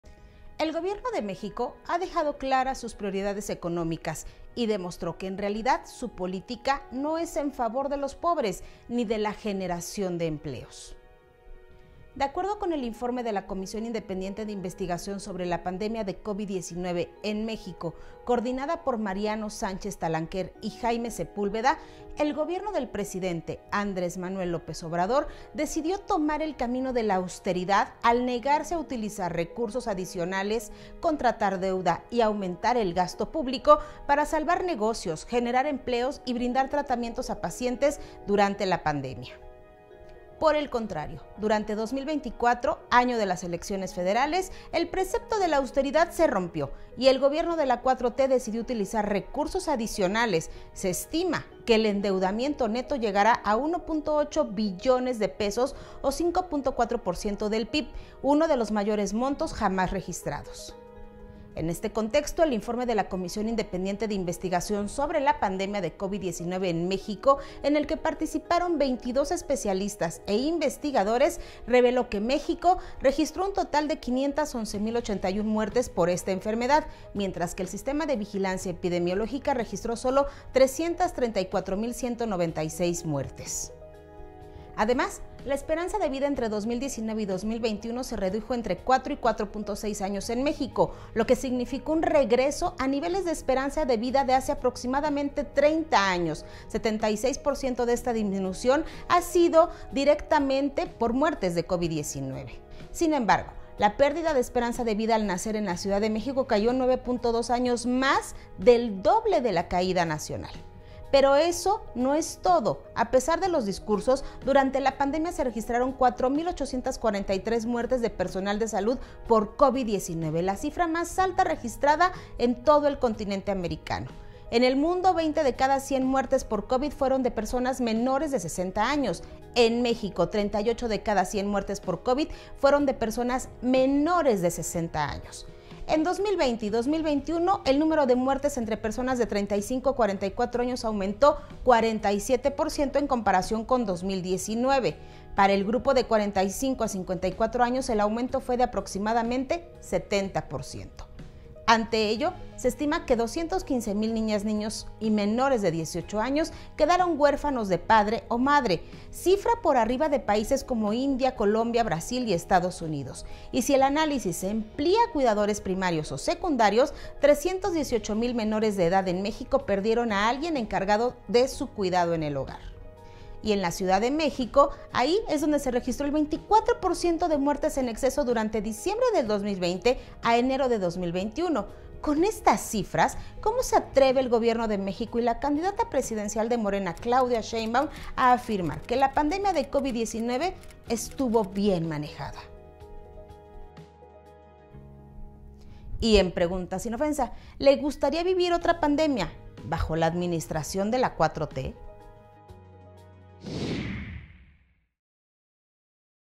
Austeridad en pandemia; despilfarro en elecciones
Austeridad en pandemia; despilfarro en elecciones. La PreguntaSinOfensa de Karina Aguilar.